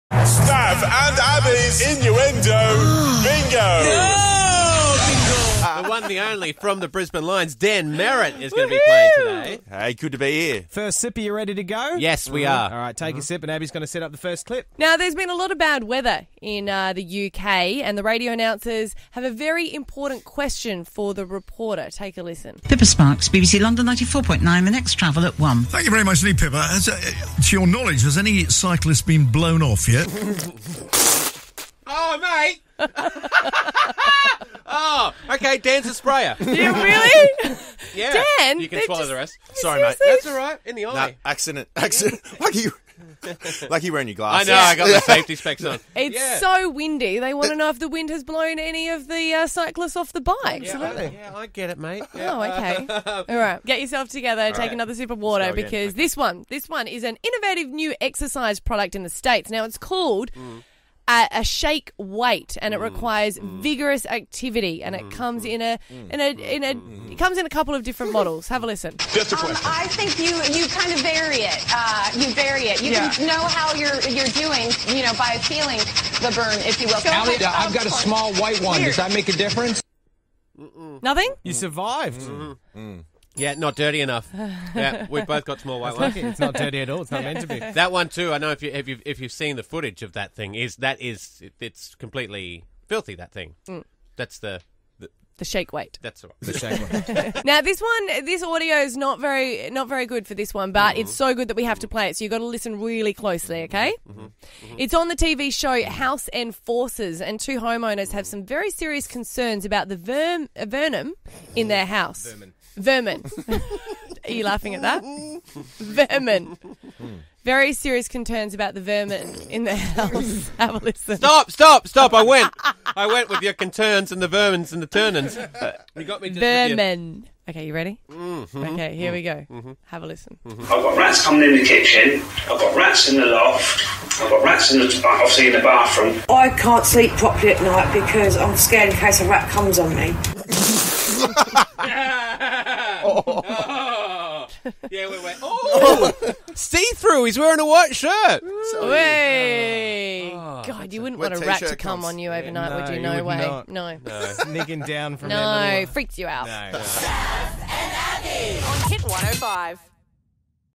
Dan Merrett plays Innuendo Bingo
Daniel Merrett visited the Hit105 studios last week and played a game of Innuendo Bingo with Stav and Abby.